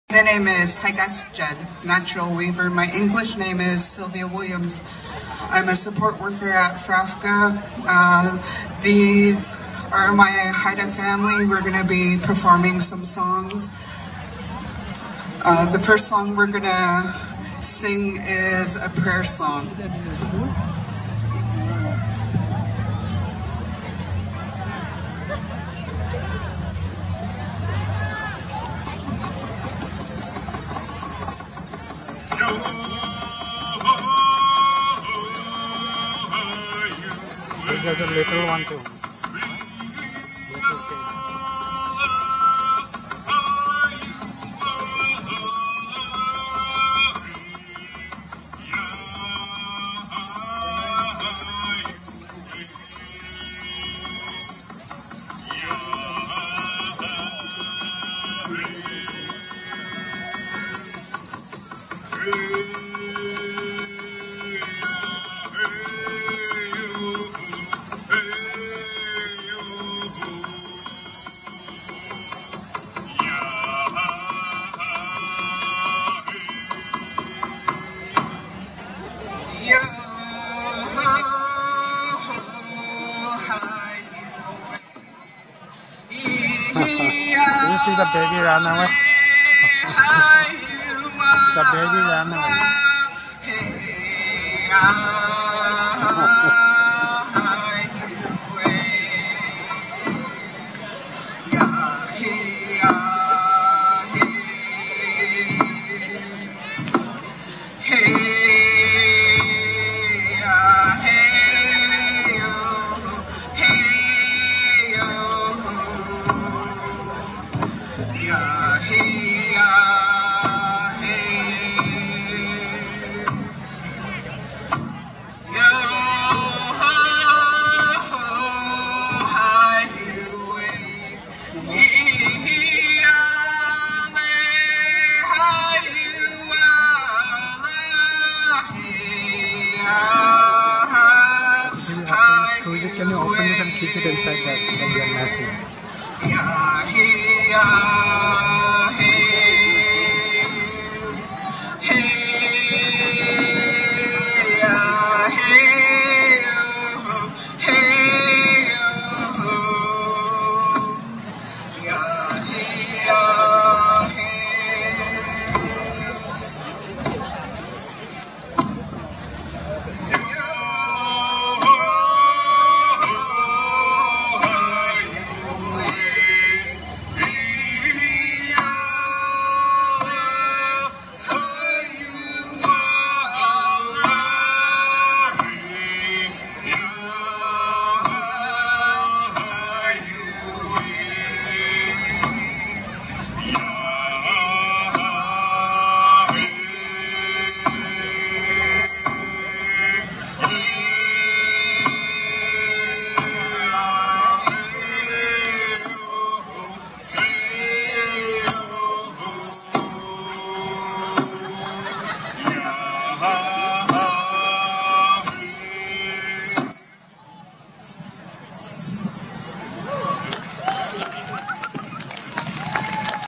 Haida Prayer song performance @ 2023 National Indigenous Peoples Day in Surrey, BC, Canada 🍁
A group of Haida artists, including a cute toddler, perform a traditional Haida Prayer song during the National Indigenous Peoples Day Celebration & Wellness Event at Bill Reid Millennium Amphitheatre in Cloverdale, Surrey, BC, Canada on Wednesday, June 21, 2023.

The event, jointly hosted by Semiahma (Semiahmoo), q̓ʷɑ:n̓ƛ̓ən̓ (Kwantlen), and q̓ic̓əy̓ (Katzie) First Nations, included cultural sharing from the local First Nations with teachings and practice through art, music, storytelling and dance.

Haida are an indigenous group who have traditionally occupied Haida Gwaii, an archipelago just off the coast of British Columbia, Canada, for at least 12,500 years. The Haida are known for their craftsmanship, trading skills, and seamanship. In Haida Gwaii, the Haida government consists of a matrix of national and regional hereditary, legislative, and executive bodies including the Hereditary Chiefs Council, the Council of the Haida Nation (CHN), Old Massett Village Council, Skidegate Band Council, and the Secretariat of the Haida Nation. The Kaigani Haida live north of the Canadian and U.S. border which cuts through Dixon Entrance south of Prince of Wales Island (Tlingit: Taan) in Southeast Alaska, United States; Haida from K'iis Gwaii in the Duu Guusd region of Haida Gwaii migrated north in the early 18th century.